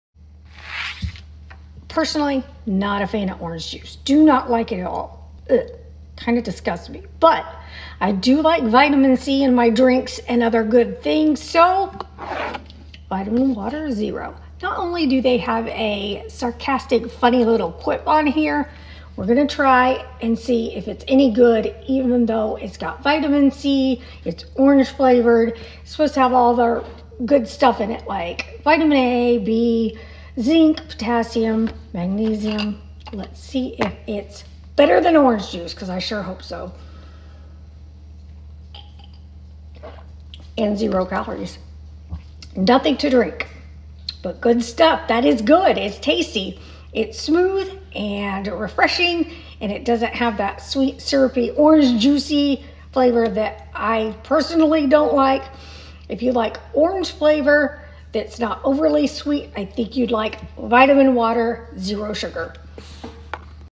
Nothing To Drink: Zero Calorie Drink Review
Zero Calorie Drink Review of Vitamin Water Zero Orange flavor